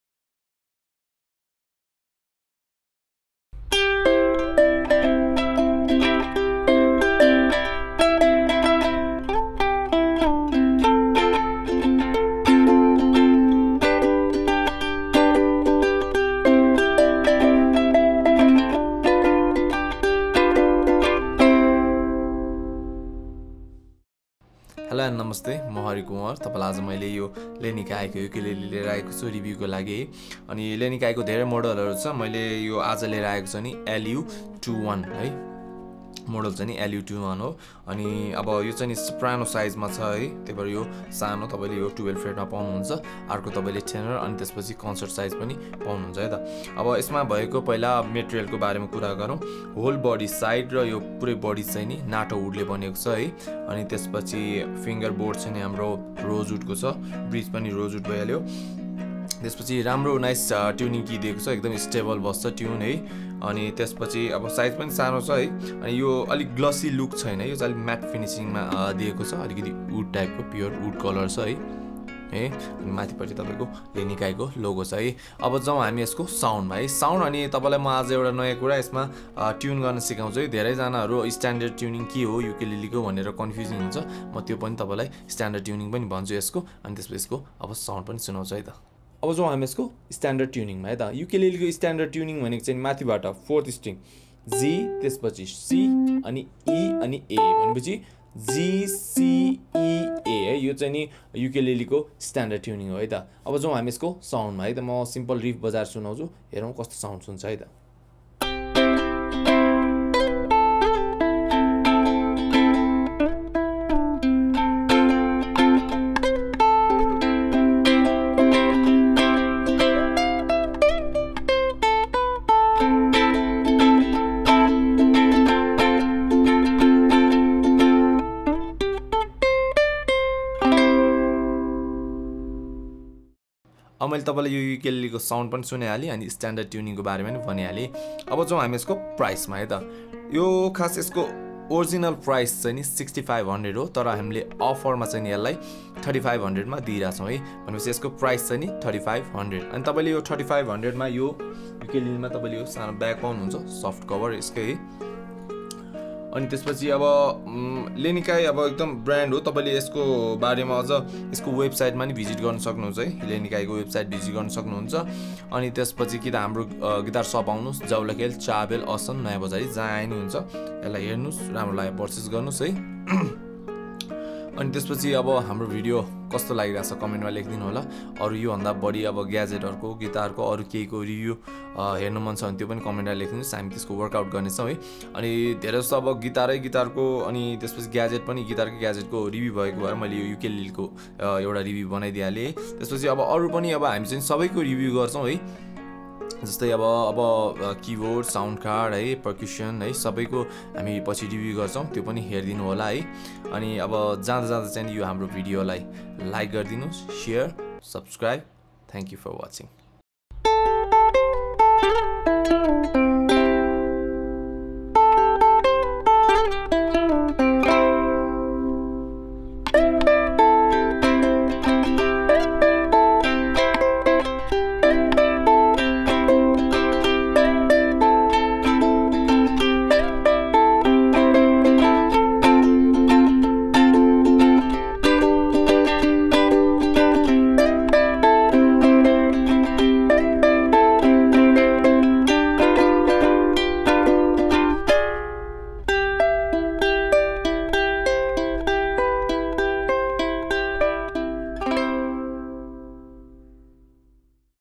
Ukulele Lanikai | LU21 | Nepali Review | Guitar Shop
The LU series represents Lanikai's most popular and affordable ukuleles in Nepal. They are a perfect first stop for acoustic multi-instrumentalists looking to expand their tonal palette or the first time player just beginning their lifelong love of music. The LU-21 Concert Acoustic-Electric Ukulele with Fishman Kula Electronics is an ideal size, compact yet still able to produce volume. It is also, great in terms of sounds and build quality. A perfect ukulele for the go.

Review by Hari Kunwar

Copyright by Guitar Shop .